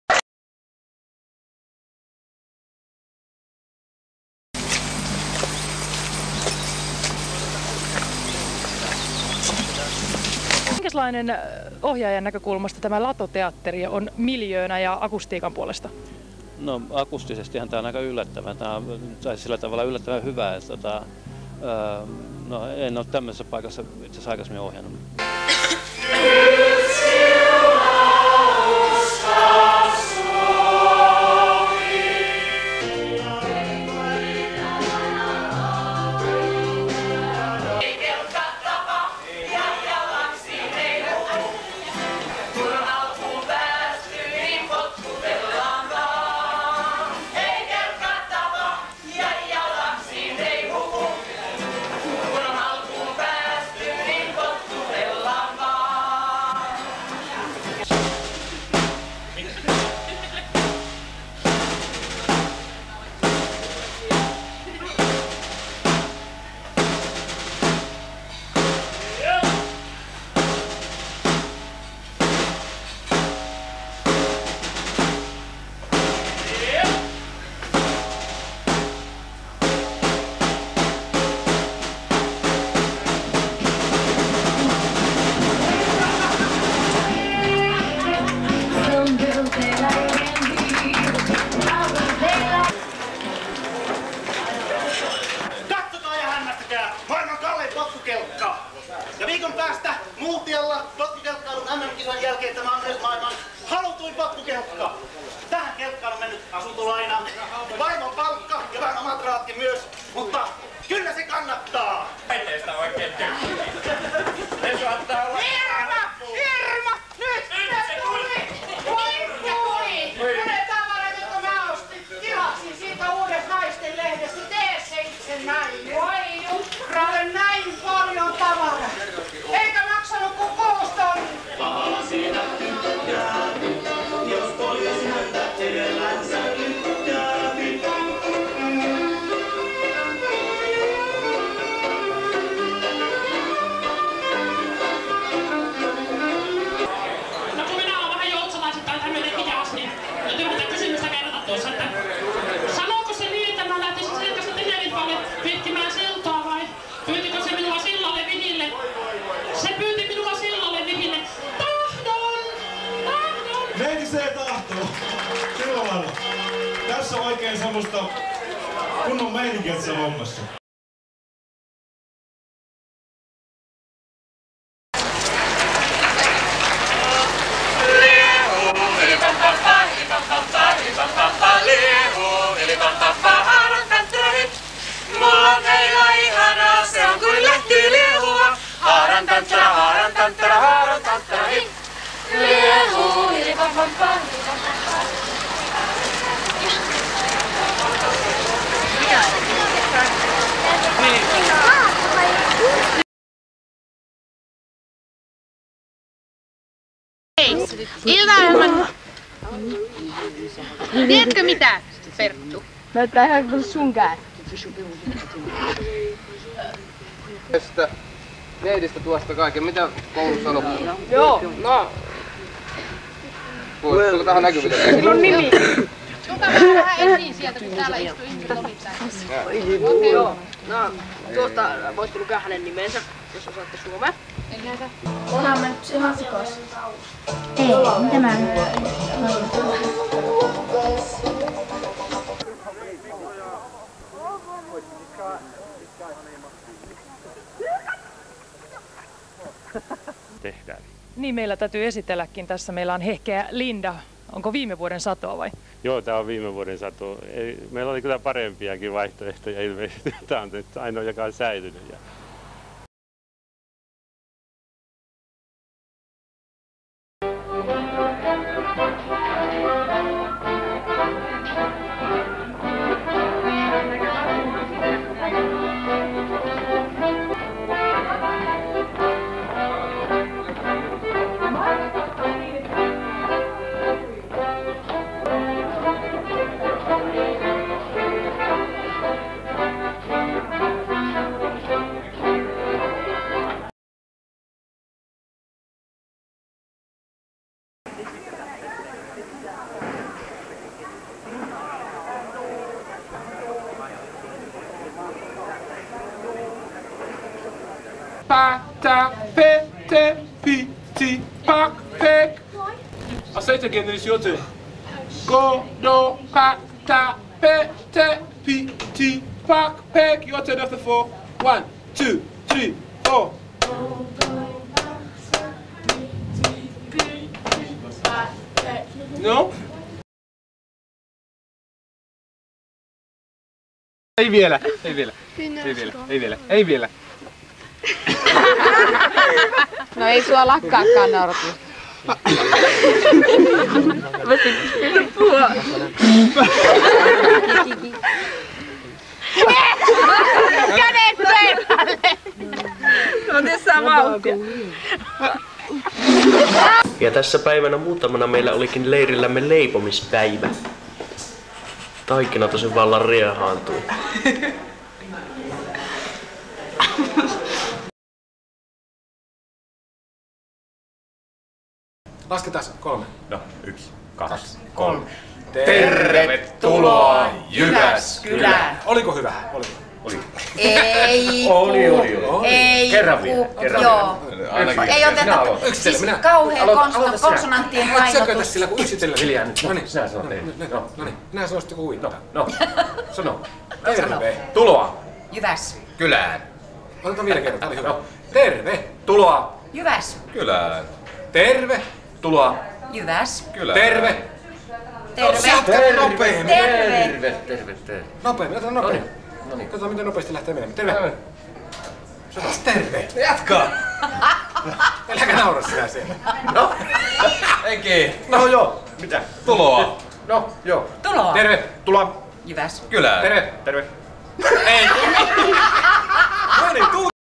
KSNSL 1980-1999 OSA 2/2
Keski-Suomen Nuorisoseurain Liiton toimintaa vuosilta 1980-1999. Osa 2/2. Videot Keski-Suomen Nuorisoseurain Liiton arkistosta.